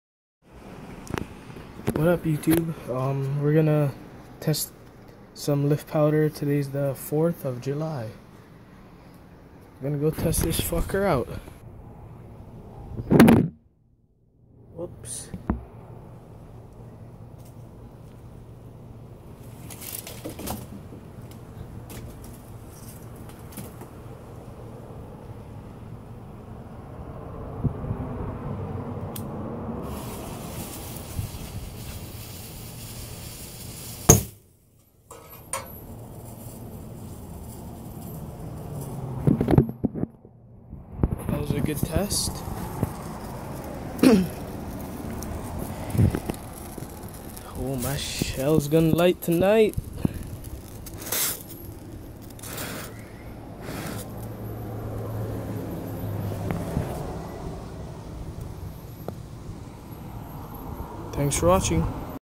Testing lift charge
Benzolift for my homemade shells for the 4th of july